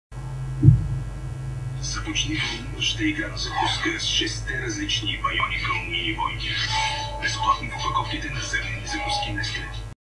2006 TV Commercial: Nestle Mini Warriors (Russian?)
TV commercial for the Mini Wariors packin line for Nestle cereal